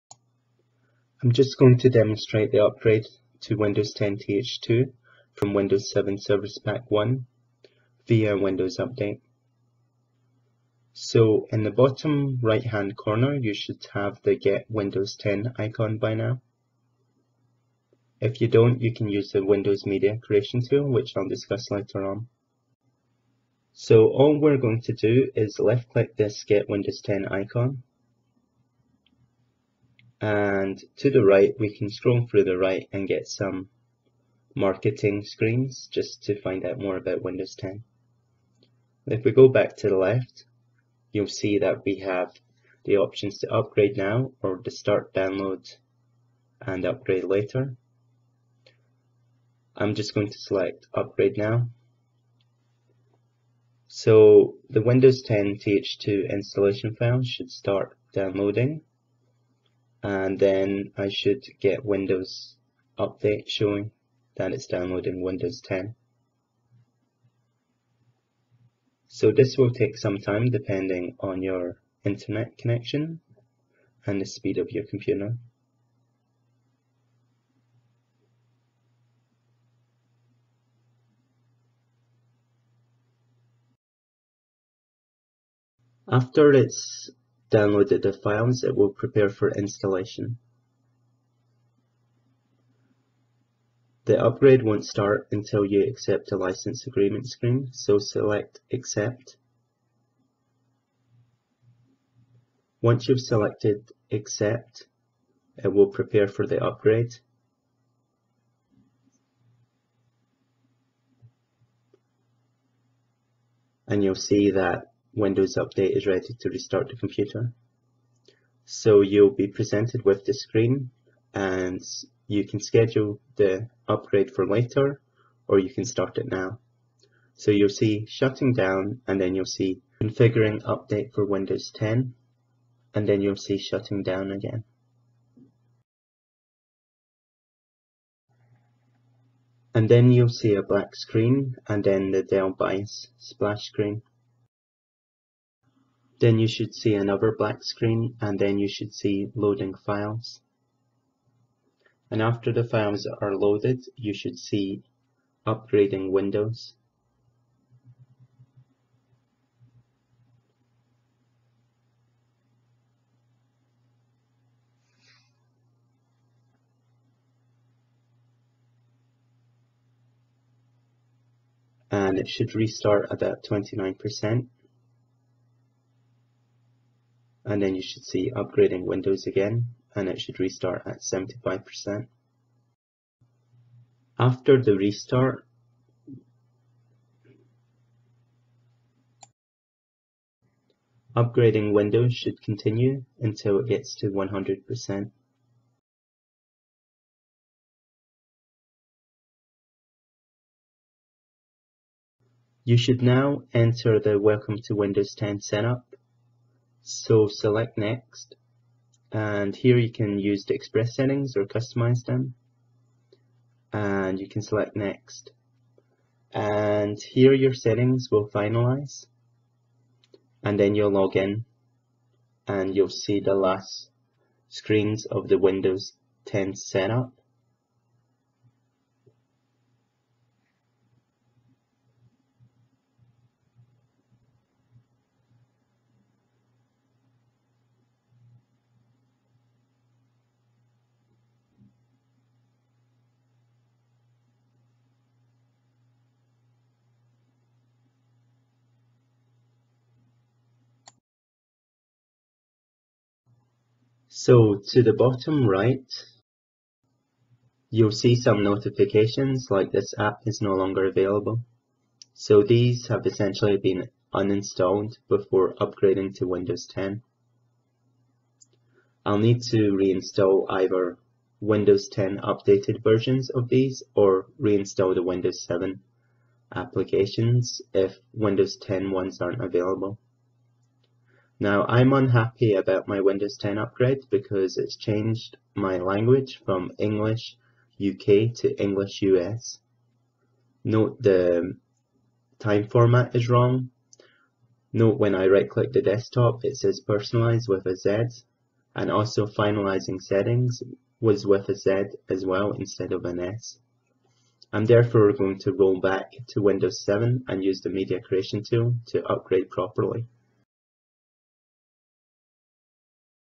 Windows 7 SP1 to Windows 10 TH2 Windows Update
Upgrading to Windows 10 TH2 from Windows 7 via Windows Update